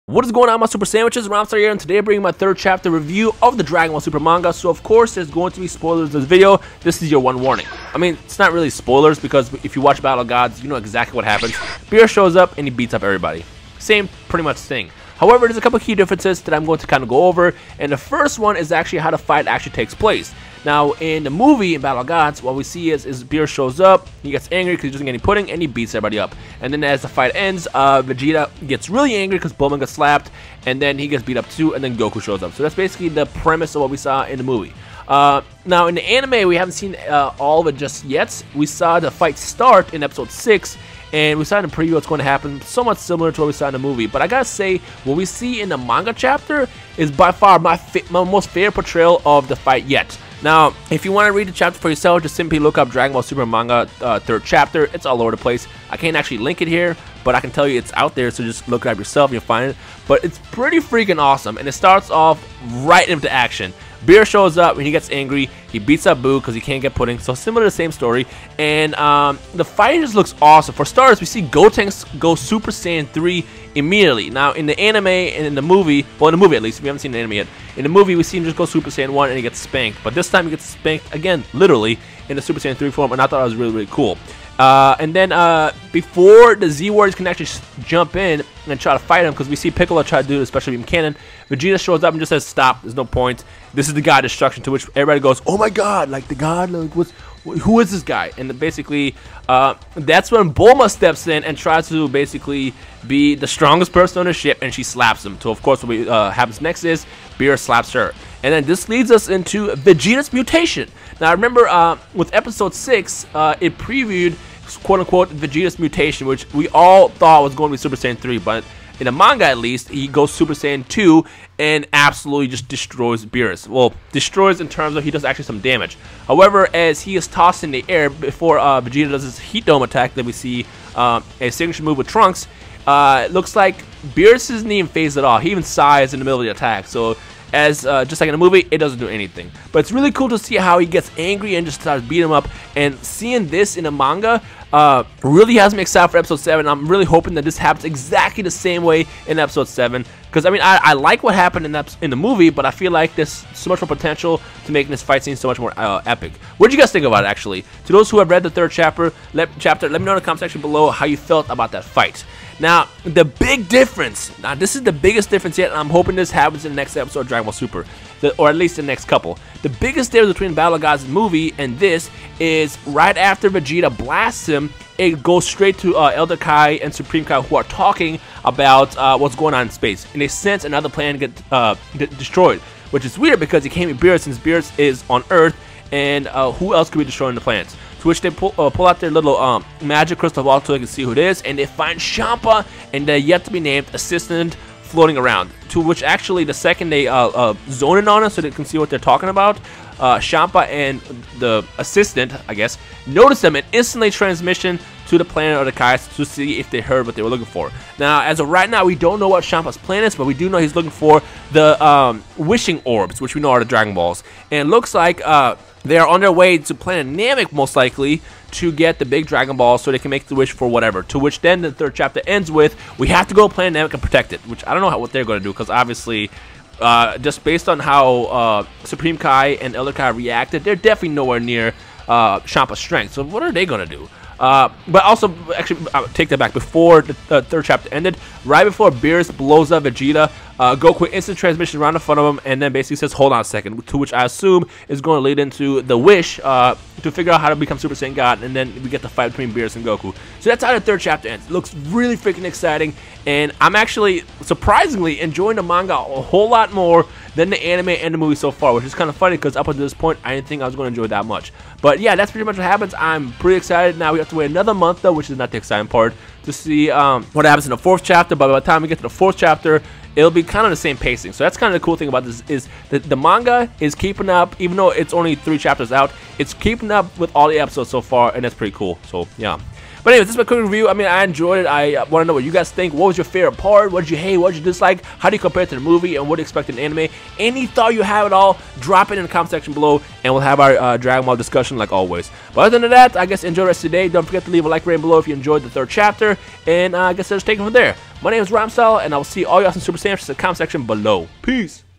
Dragon Ball Super Chapter 3 Manga Review: Beerus vs Z-Warriors, SSJ2 Vegeta, & Champa?!
Dragon Ball Super Chapter 2 features the Z-Warrior fight against Beerus and then ends with a surprise appearance by Champa!